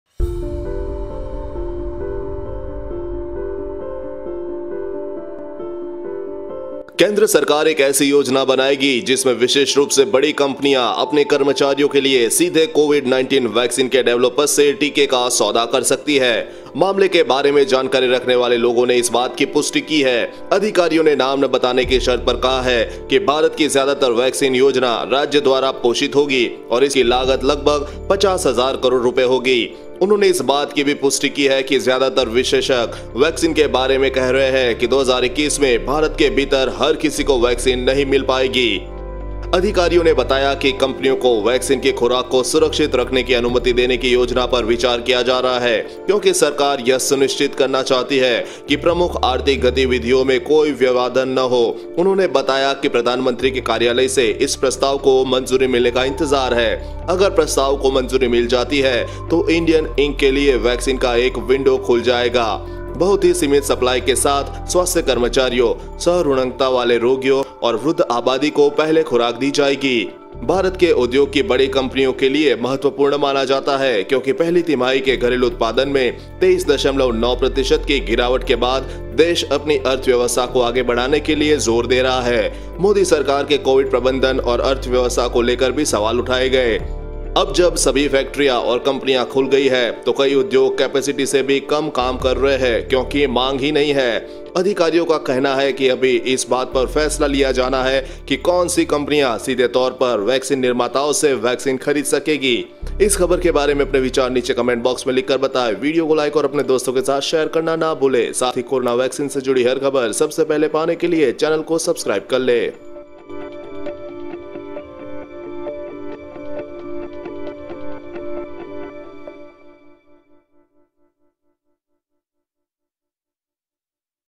भारत में बड़ी कंपनियां अपने कर्मचारियों के लिए सीधे खरीद सकेंगी Corona की Vaccine !
Coronavirus : Corona Vaccine Companies Will Be Able To Get Vaccines For Their Employees Directly From Vaccine Manufacturers Vaccine Distribution | Corona Vaccine Latest News & Updates

EDITOR ► Manjeet